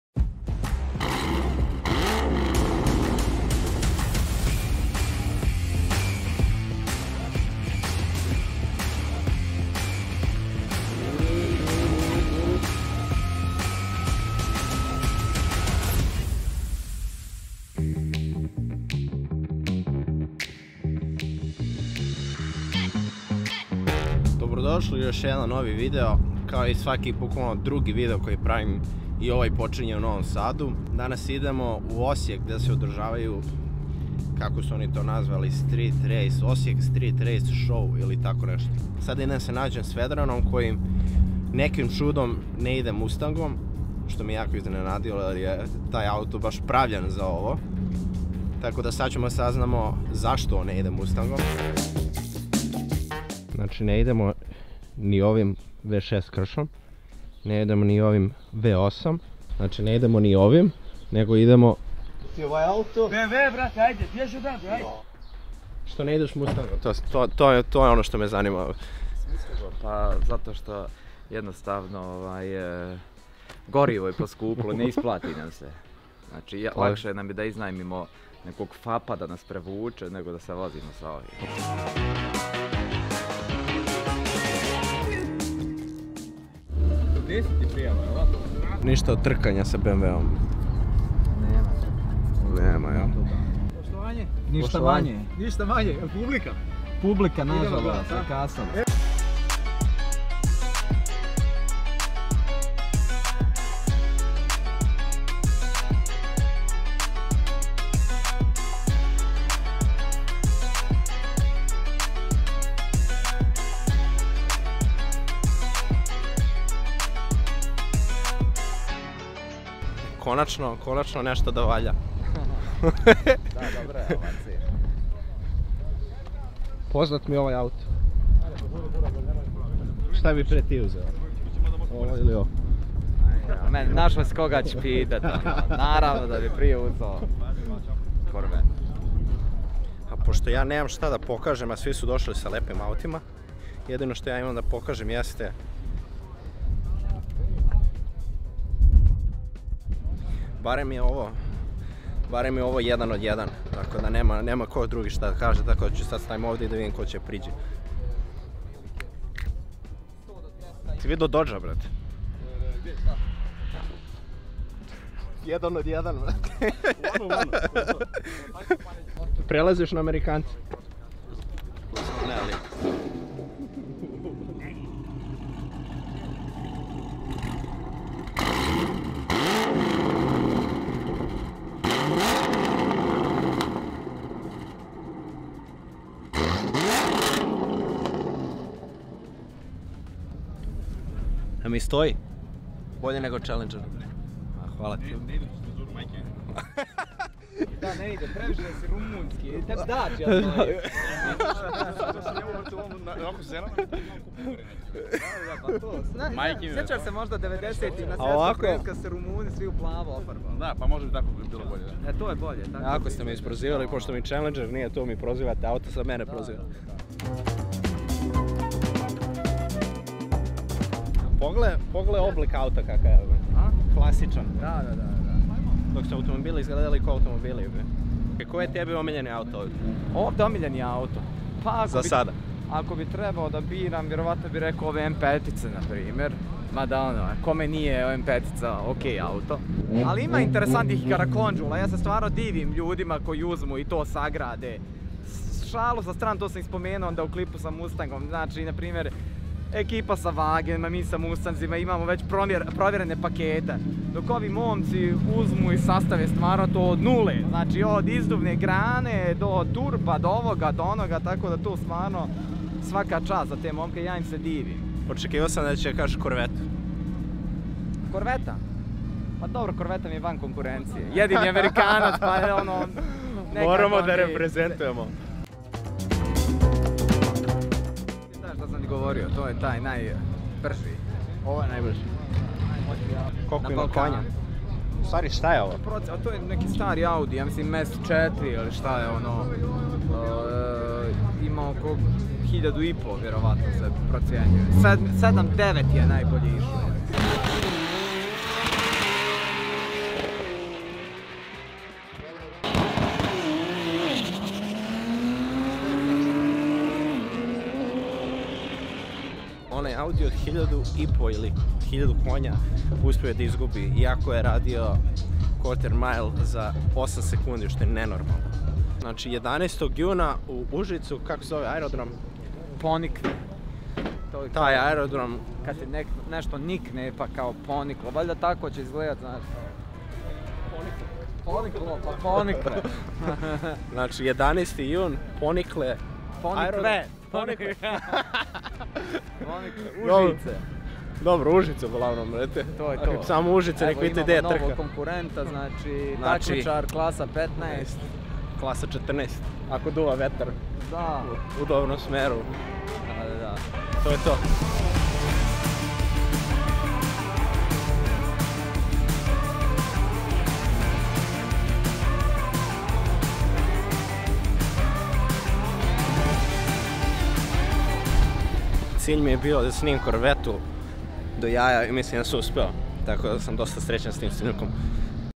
Osijek Street Race 2022 Vlog
Posetili smo ovogodisnji Osijek Street Race Show - Trke ubrzanje i ovako smo se proveli.